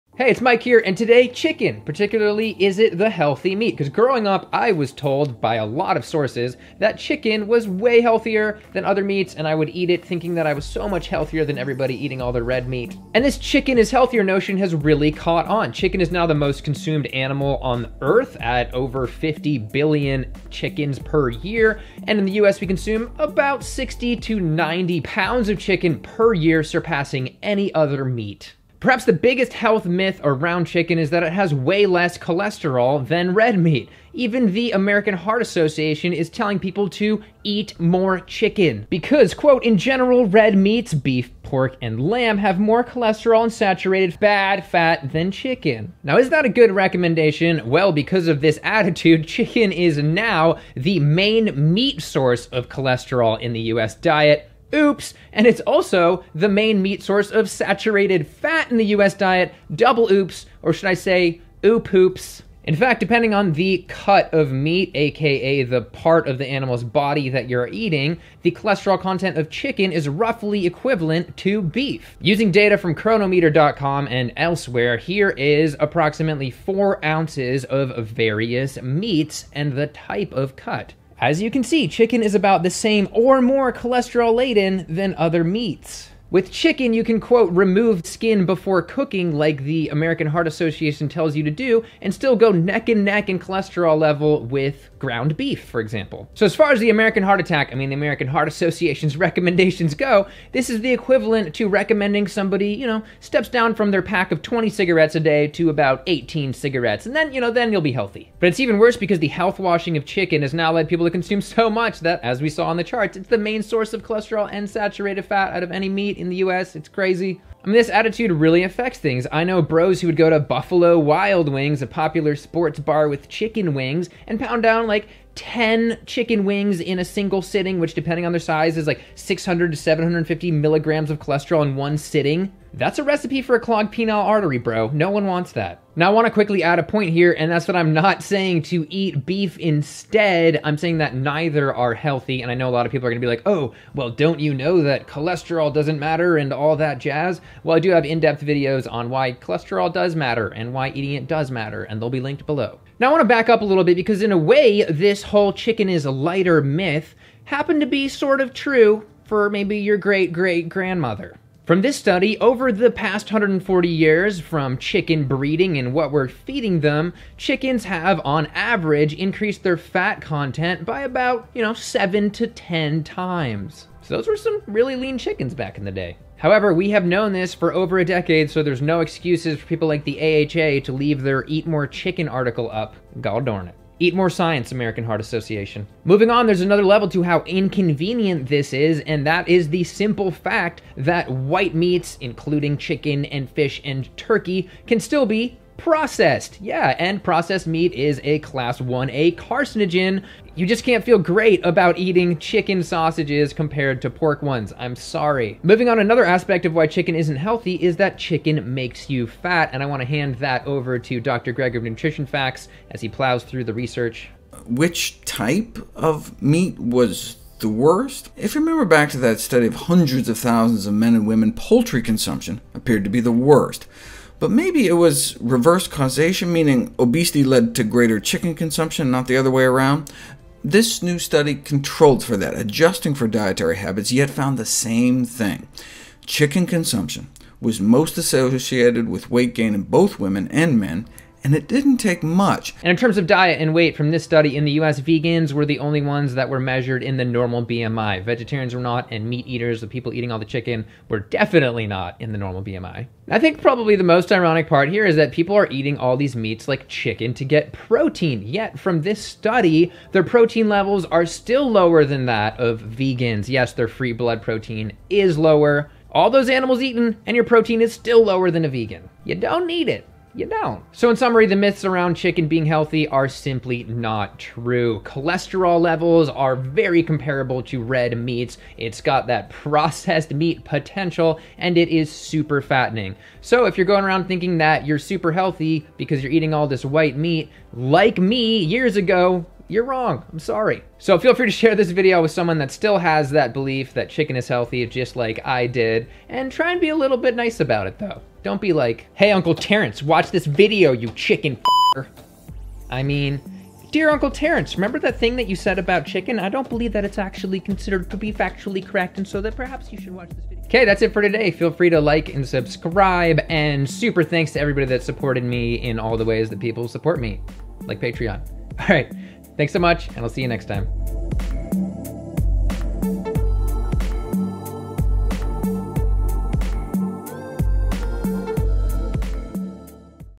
American Heart Association: "Eat More Chicken"
An evidence-based examination of whether or not chicken really is the healthy meat like we are told.

Cholesterol numbers from Cronometer.com
More cholesterol numbers: